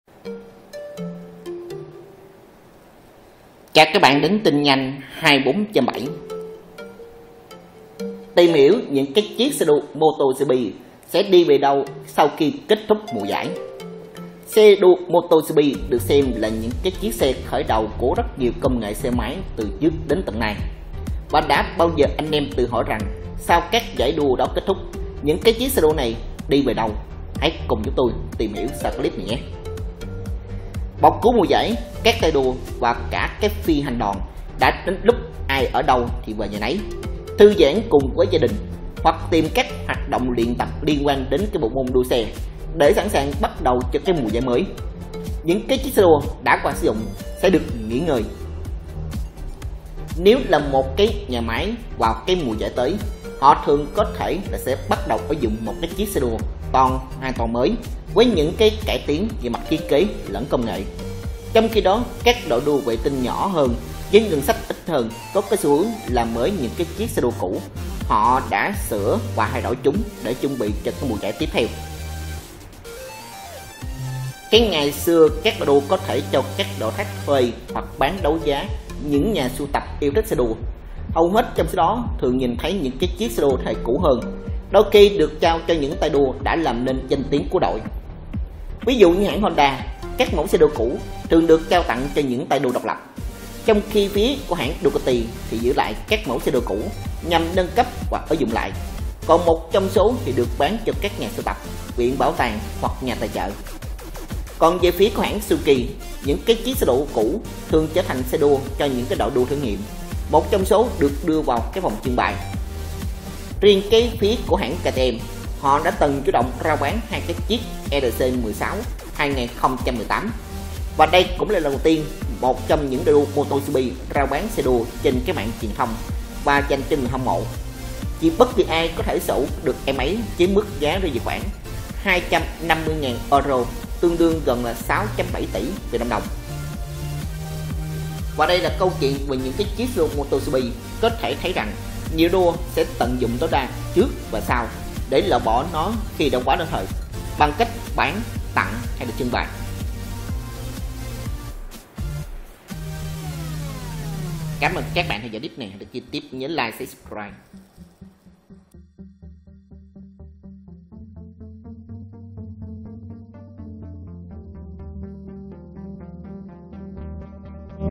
Tin nhanh 24/7 - Tìm hiểu những chiếc xe đua MotoGP sẽ đi về đâu sau khi kết thúc mùa giải.
Xe đua MotoGP được xem là những chiếc xe khởi đầu của nhiều công nghệ xe máy từ trước đến nay. Và đã bao giờ anh em tự hỏi rằng sau khi mùa giải đó kết thúc? Những chiếc xe đua này sẽ đi về đâu? Cùng tìm hiểu clip sau đây.